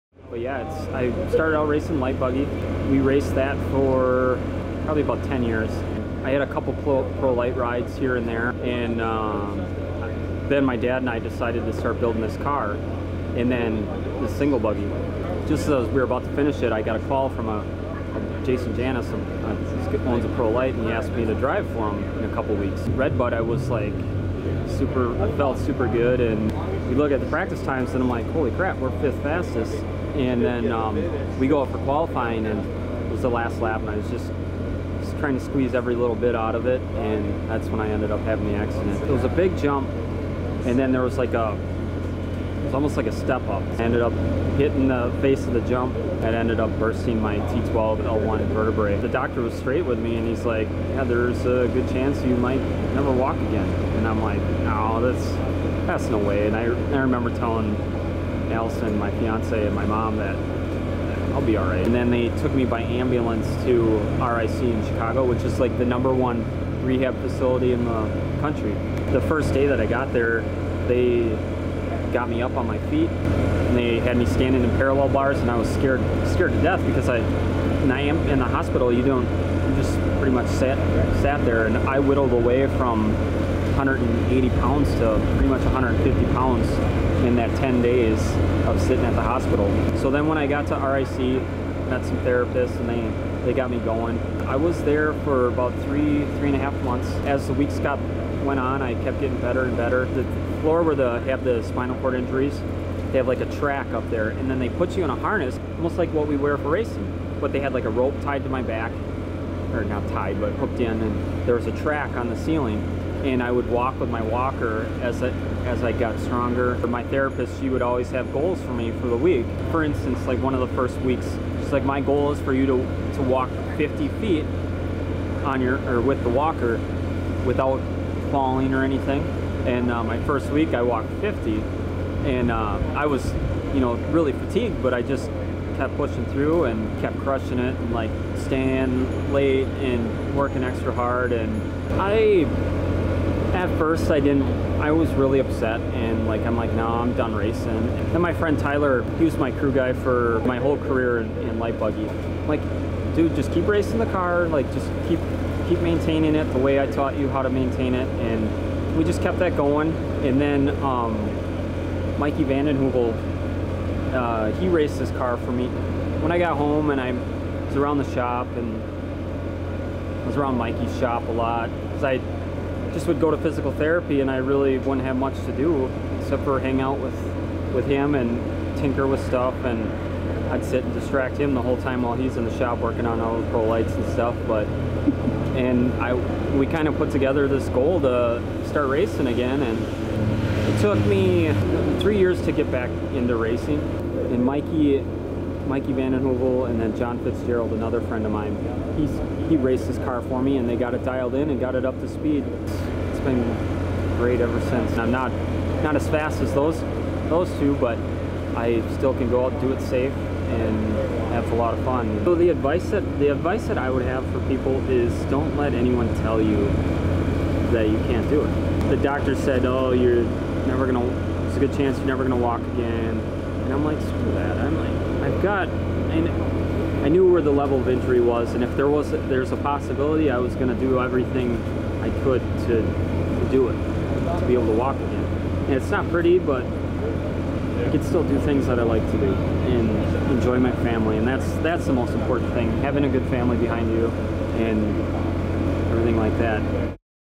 TORC - Matt Gerald Recovers, Returns to RedBud
For one sportsman series Single Buggy racer, TORC: The Off-Road Championship Series Rumble at RedBud has special meaning. Five years ago at this venue, Matt Gerald’s life changed forever. In the blink of an eye, Gerald’s racing career and work off the track was brought to a sudden halt. What happened at RedBud would redefine not only his outlook on racing, but life itself. And although it may have seemed Gerald’s 2012 RedBud race might have been his final stop, he has overcome insurmountable odds to return to racing and will join the TORC Sportman driver on the track again in 2017.